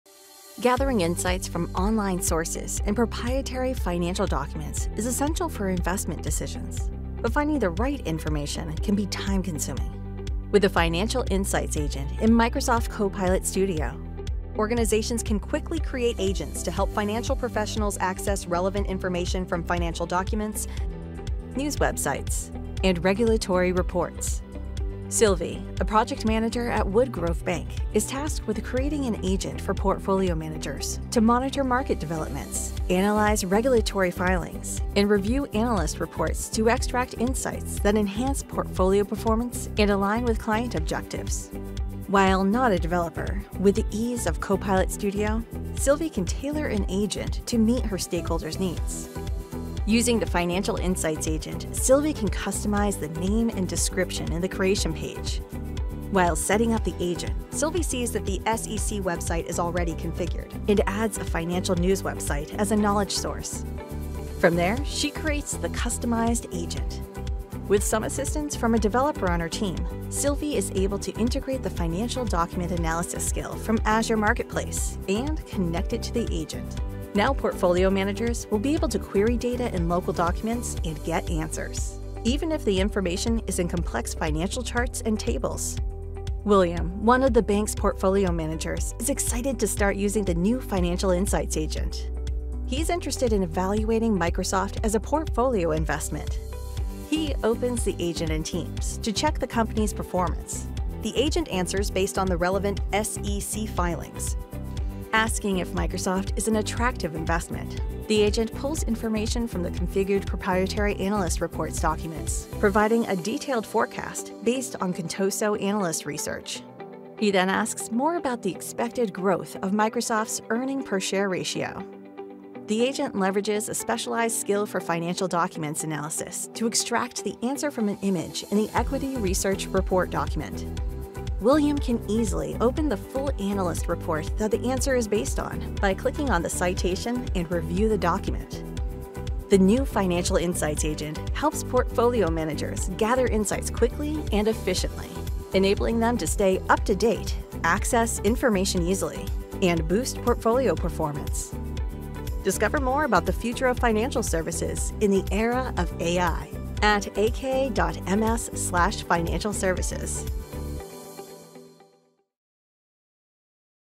Unlocking Financial Insights with Innovative AI Solutions
Discover how financial professionals can optimize their workflow with the Financial Insights Agent, available in Microsoft Copilot Studio. Create a customized agent that provides real-time insights from financial documents, news websites, and regulatory reports to enhance portfolio performance.